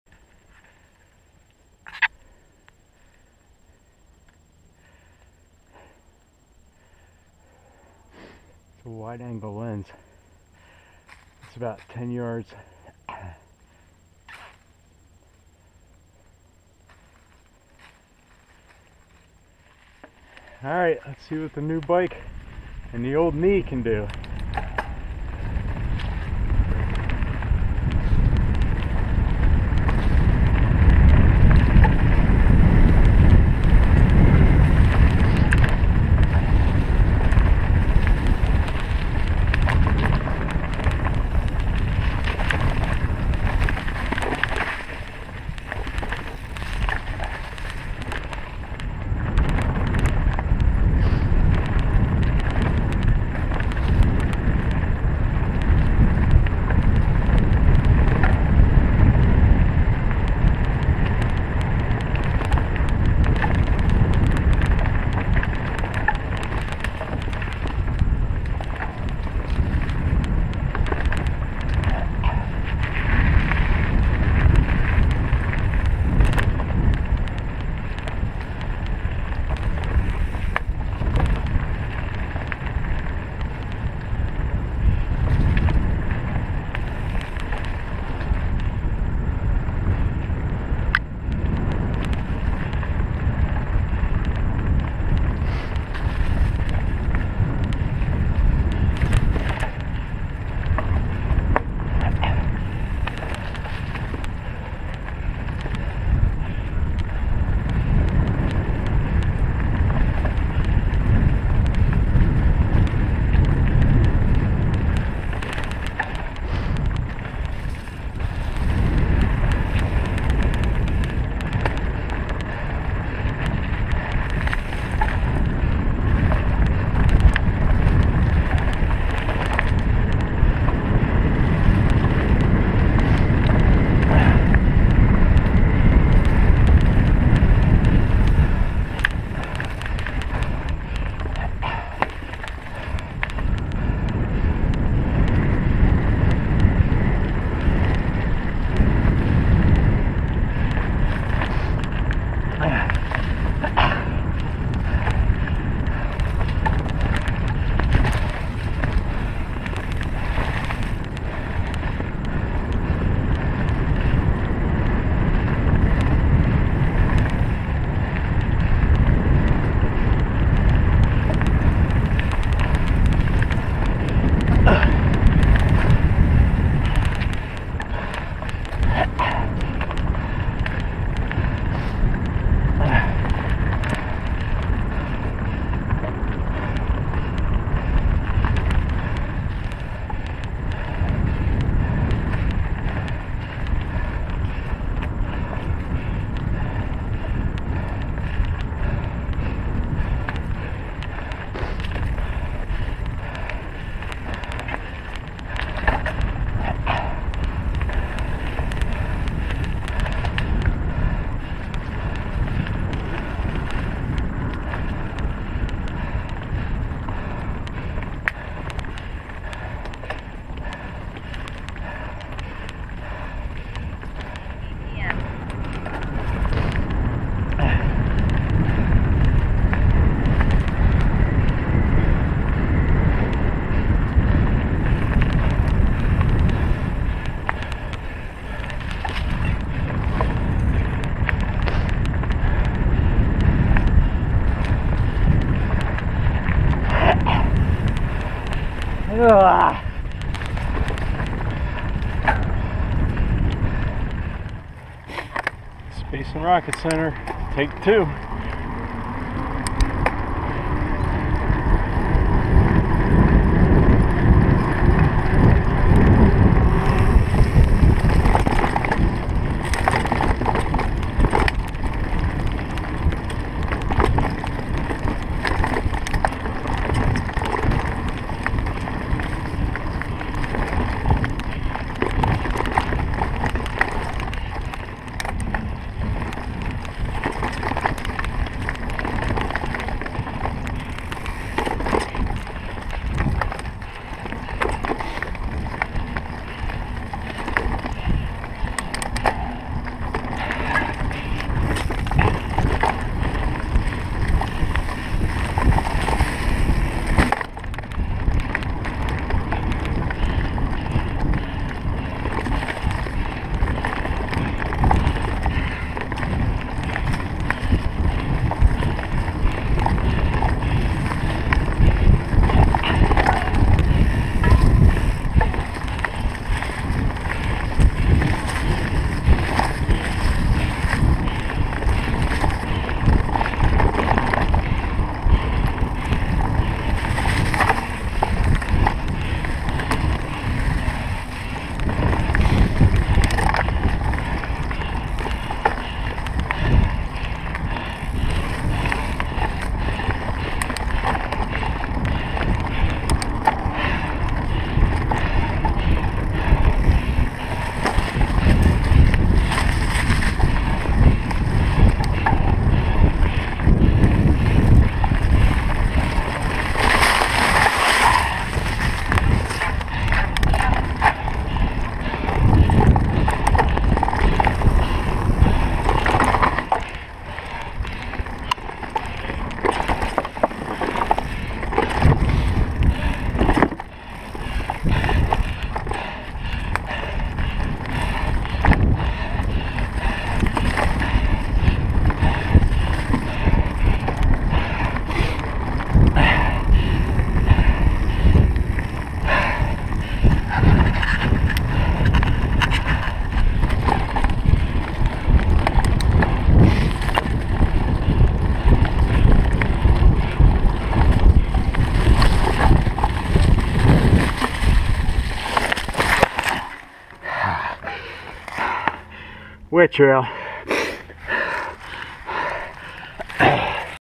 Weekend MTB Highlights
Doesn't show speed or elevation, but it was a fun experiment. Clips from Monte Sano Family Trail and Space and Rocket Center Trail Cockpit Section